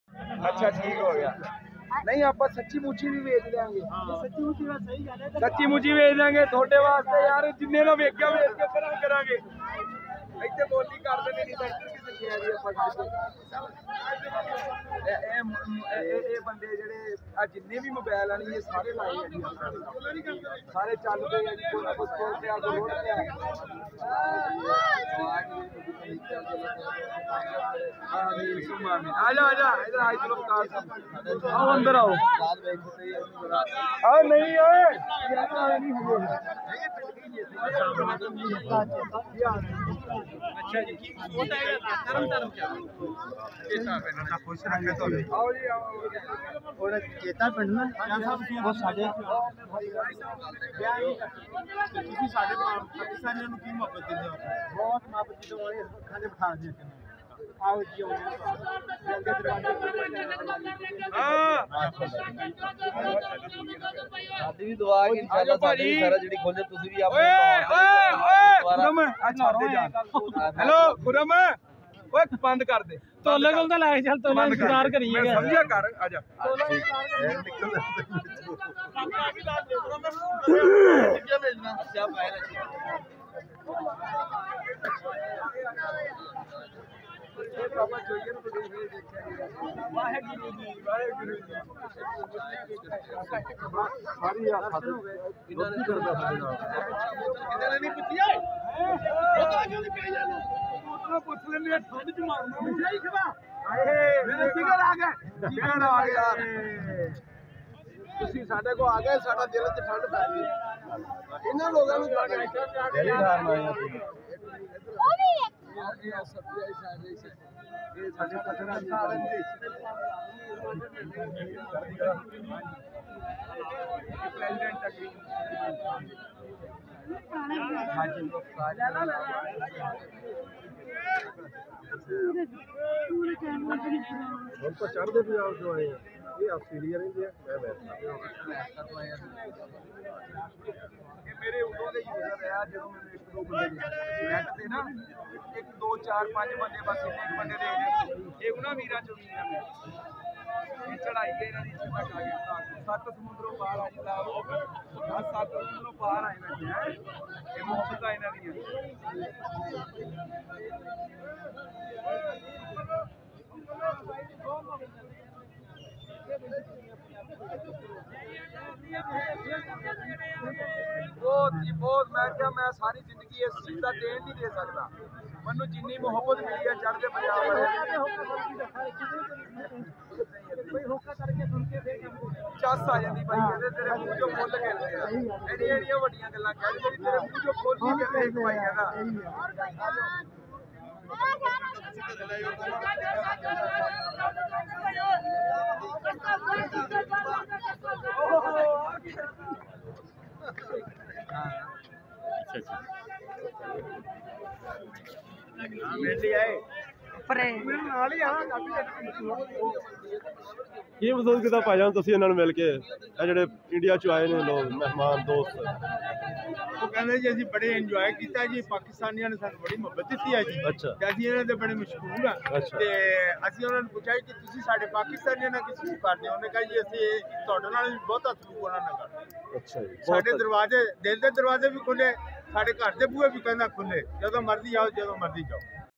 Indian Sikh Sardar Visit Chishtian Fruit Mandi Very Emotional Video
Indian Sikh Sardar Visit Chishtian Fruit Mandi,
Indian Sikh Sardar Visit Chishtian Fruit Mandi  Video,